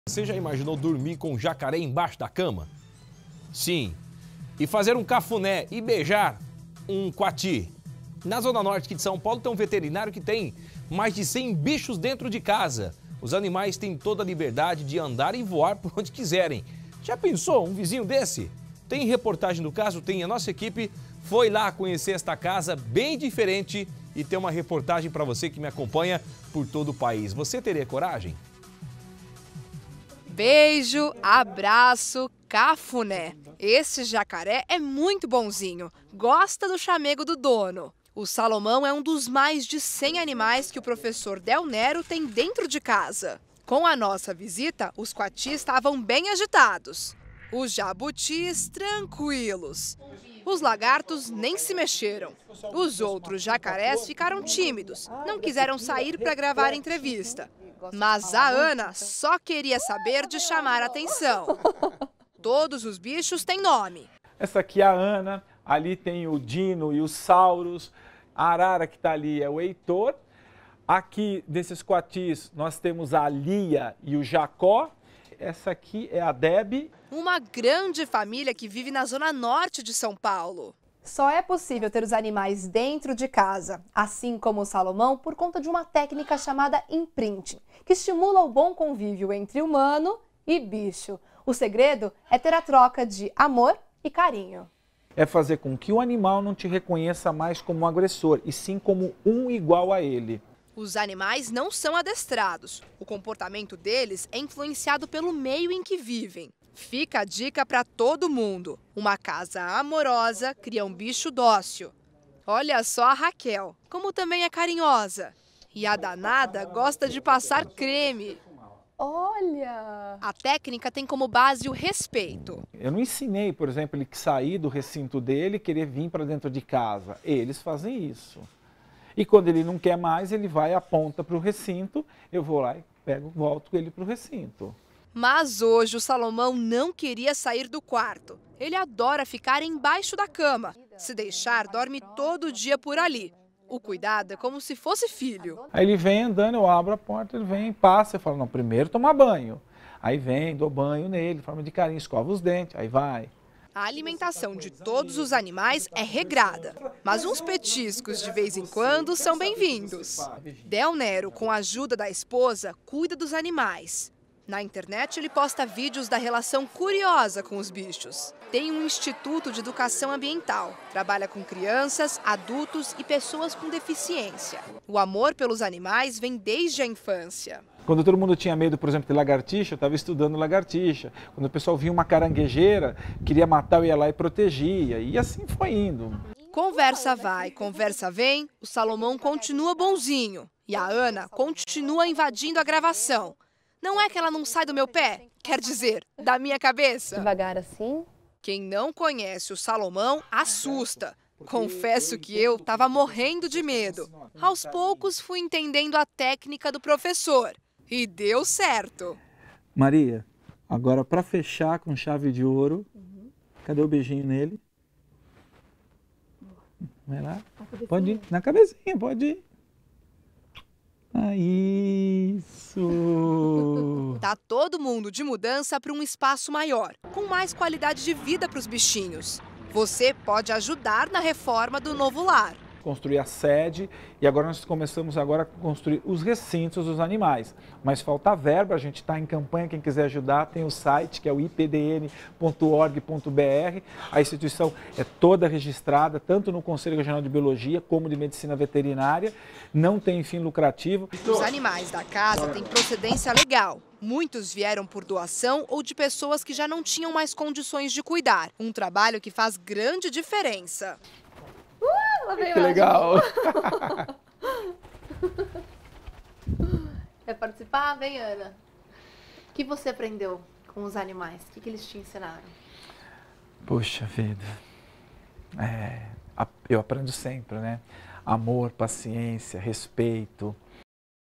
Veterinário cria jacaré dentro de casa
Você já imaginou dormir com um jacaré embaixo da cama? E fazer cafuné e beijar um quati? Na zona norte de São Paulo tem um veterinário que tem mais de cem bichos dentro de casa. Os animais têm toda a liberdade de andar e voar para onde quiserem. Veja na reportagem. Para assistir ao conteúdo na íntegra, acesse playplus.com